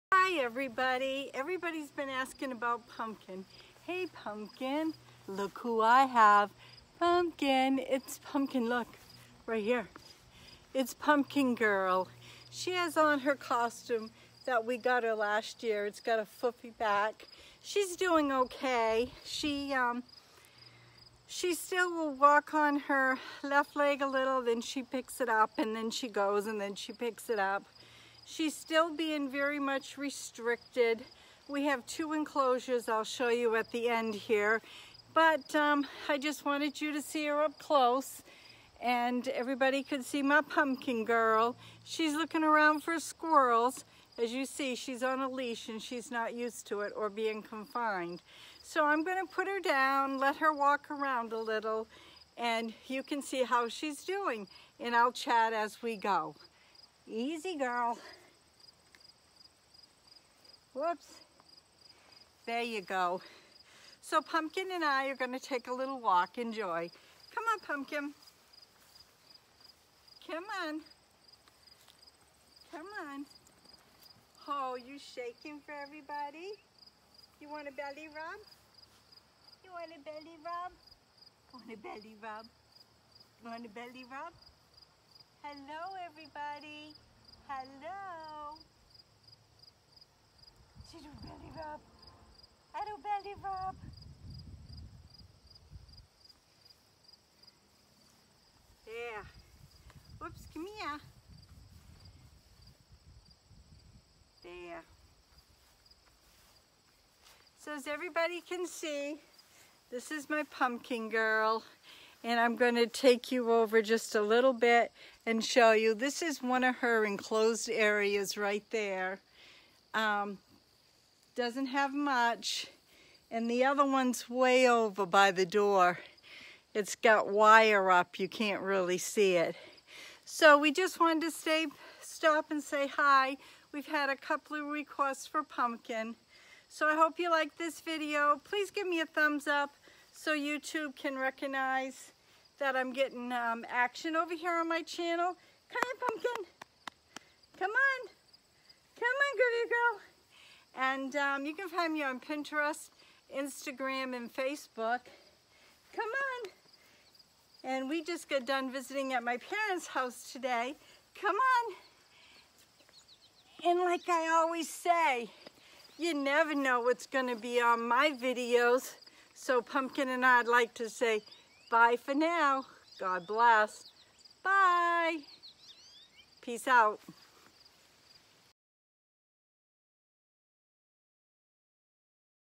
Come Greet And Meet Pumpkin 🎃
This here is Pumpkin she is six years old she turned six years a couple of weeks ago .. She is a good girl very spoiled wouldn’t want it any other way ..